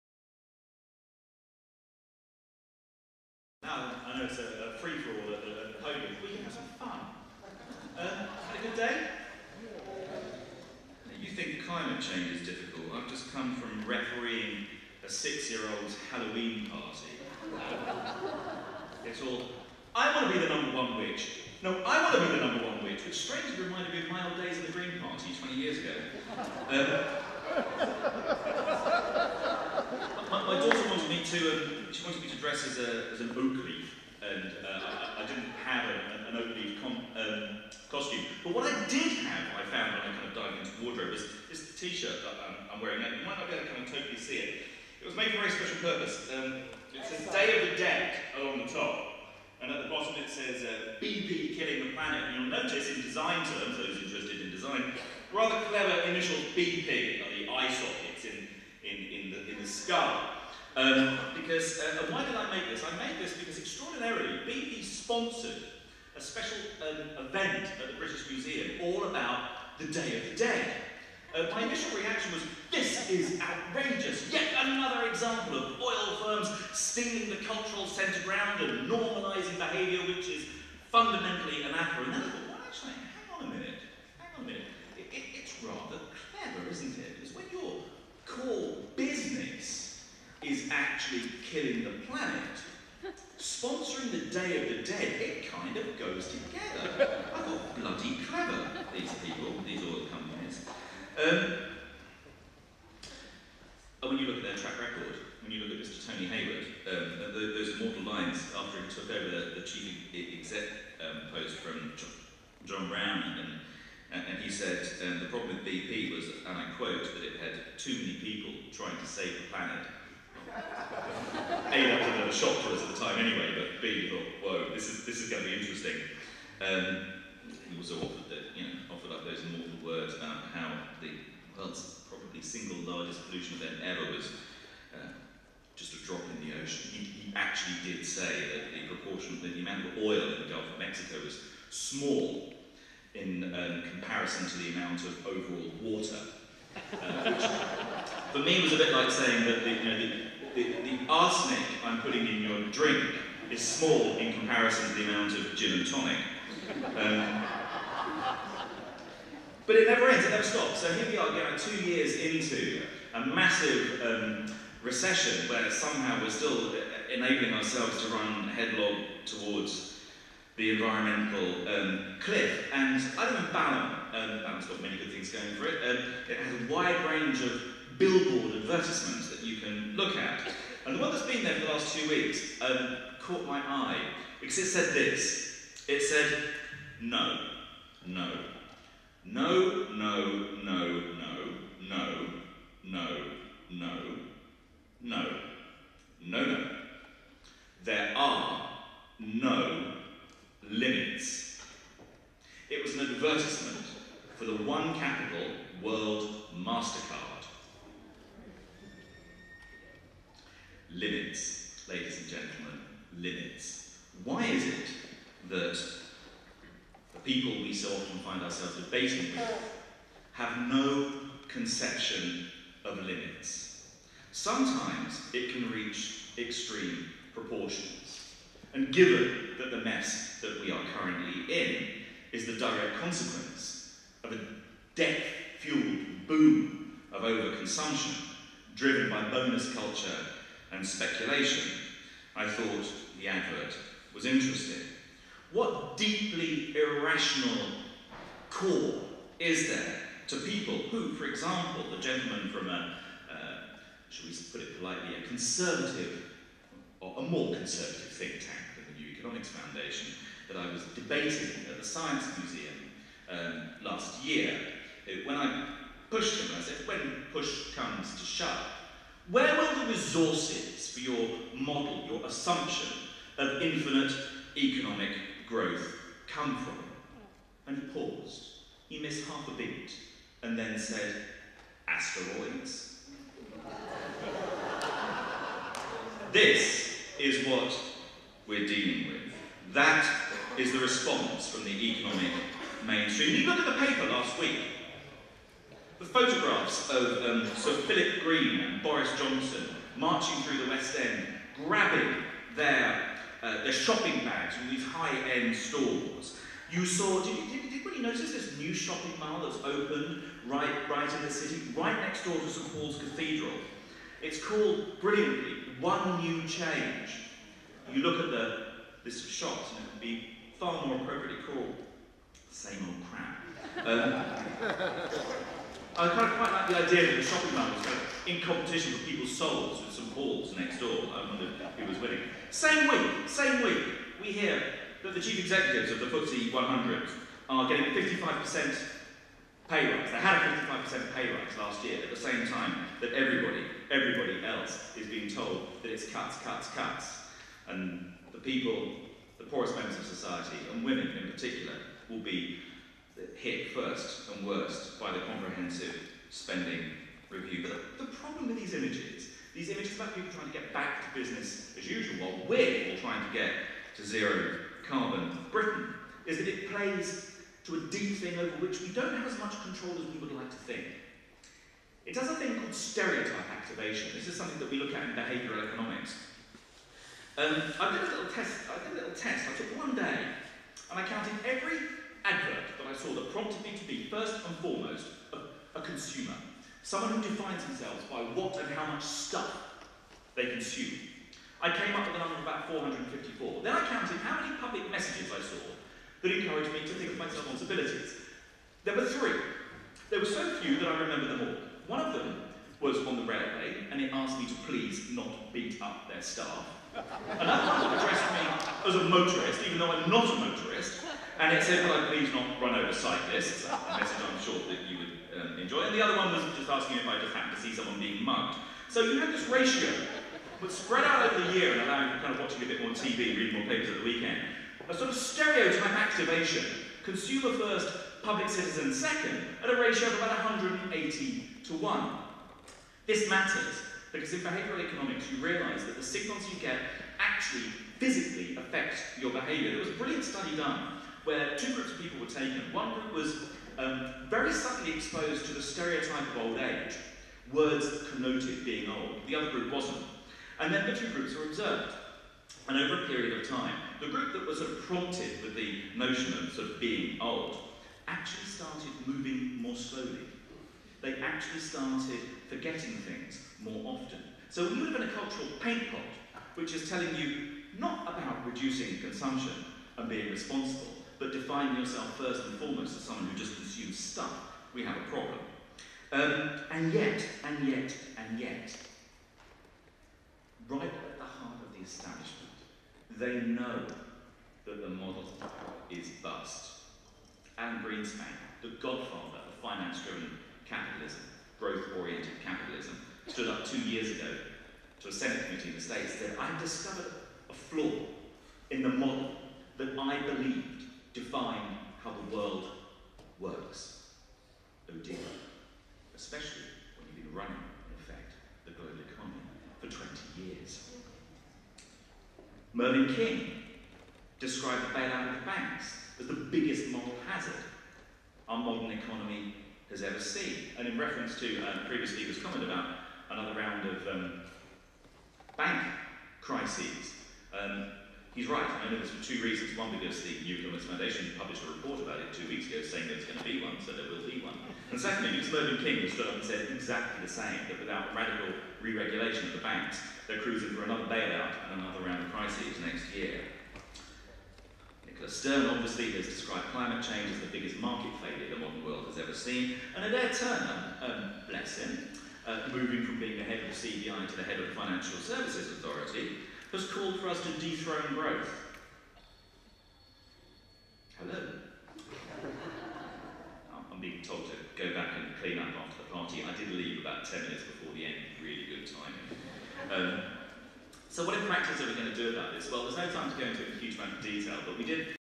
Andrew Simms from the New Economics Foundation on consumerism
Part of Campaign against Climate Change's public meeting "Zero Carbon by 2030", at Friends House, Euston, London on 30th October 2010